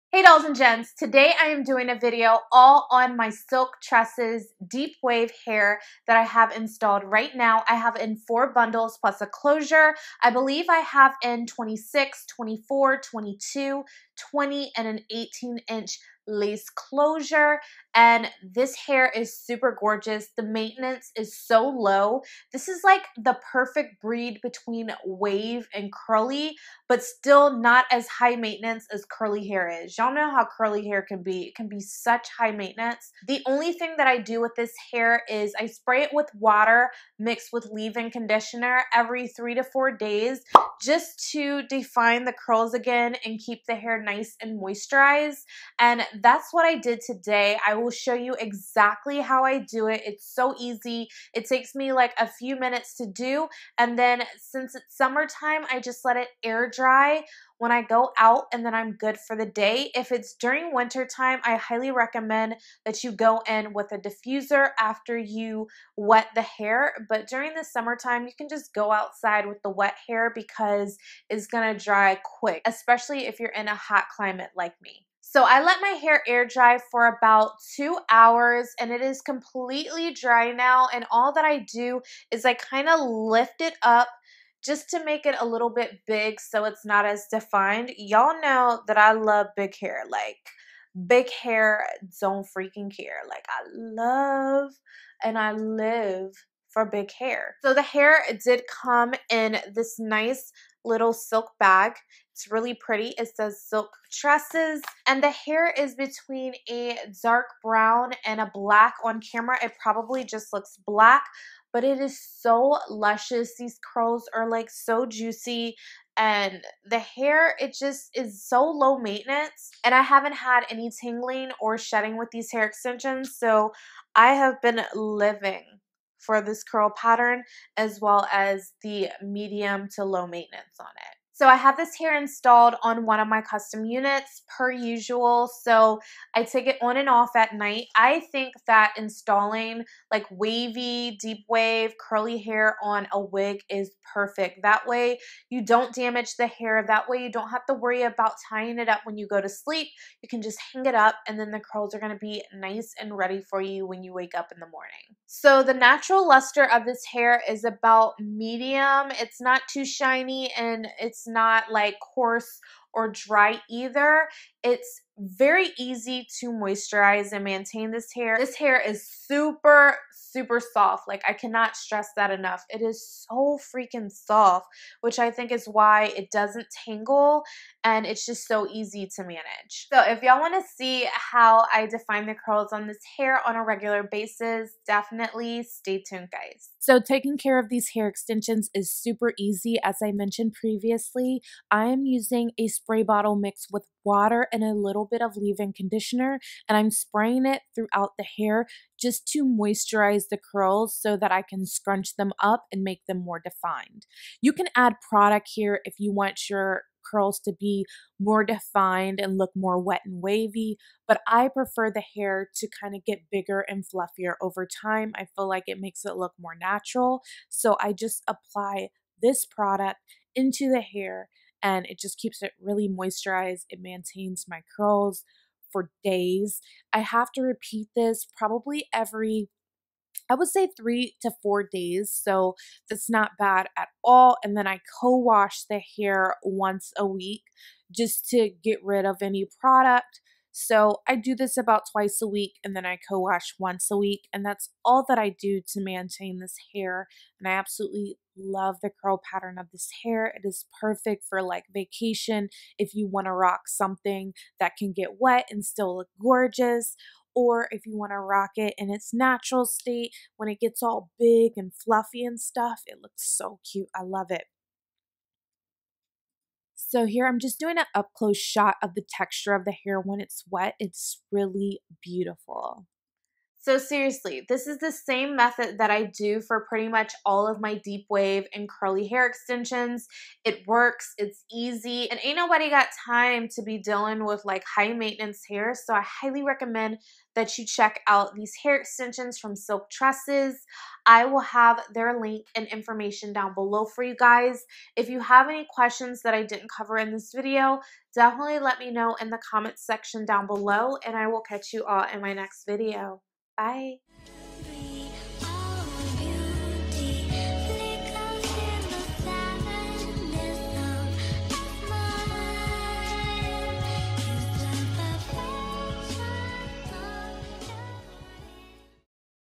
Perfect Summer Curls! | Silk Tresses Hair Tutorial + Review | Makeupd0ll
► T H U M B S  U P to show your support! =)
► W A T C H M Y L A S T E S T V I D E O S

Hi loves!  Wavy and Curly hair extensions are my essentials for summertime!  Today, I'm sharing how easy it is to style my Silk Tresses Deep Wave hair extensions.  Enjoy! xo

► Hair Info: Silk Tresses Deep Wave Hair Extensions in 26, 24, 22, 20 & 18-inch lace closure

B O O K  M E

Silk Tresses provided these hair extensions for honest review.  All opinions expressed are honest and based on my own experiences.